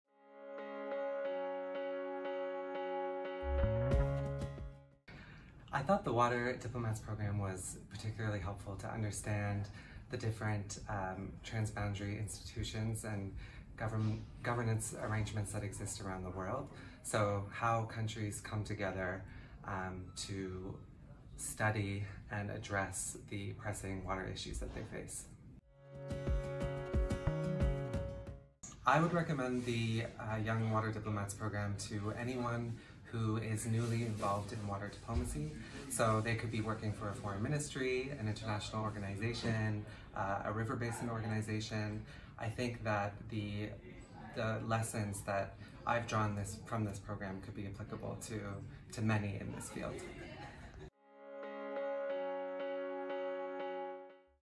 Young Water Diplomats programme - 2024
In a world where water security is becoming increasingly critical, the water diplomacy skills gained by the 18 participants in the recently concluded IHE Delft Young Water Diplomats programme are critical. Hailing from 13 different countries stretching from Finland to Zimbabwe and from Canada to Kazakhstan, the diplomats spent the past six months combining in-depth study and practical experience.